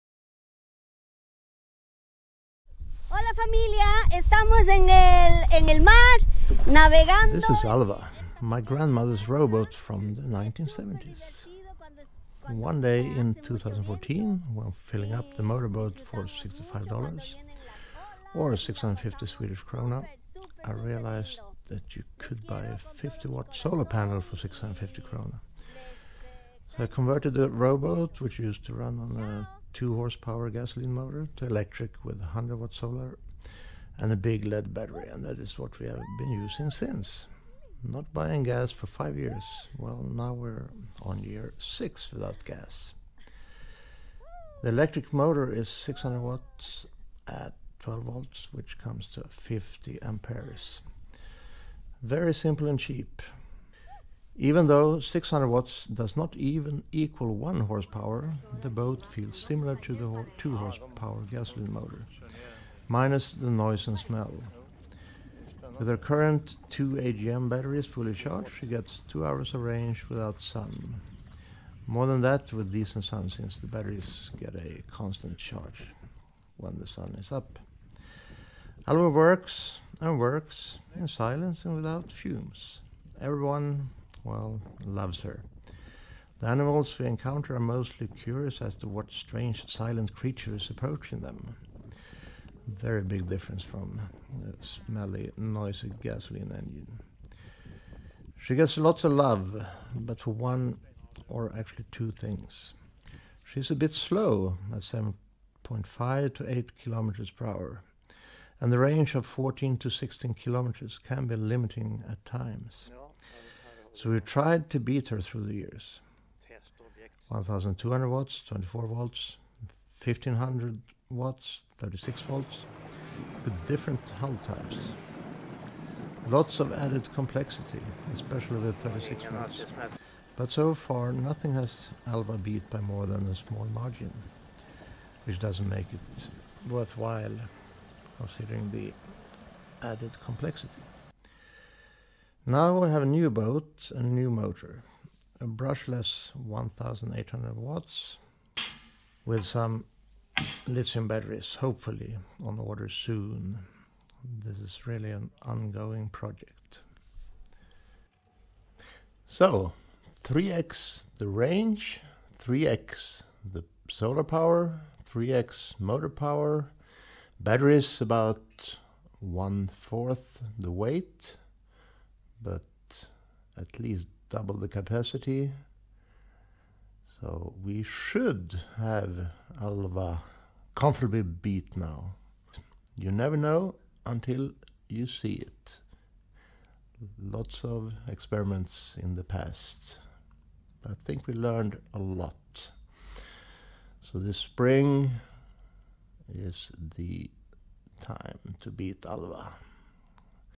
The Solar Boat Project
Detailing the quest to beat Alva, the original Solar Boat that managed to get (almost) everything right in 2014.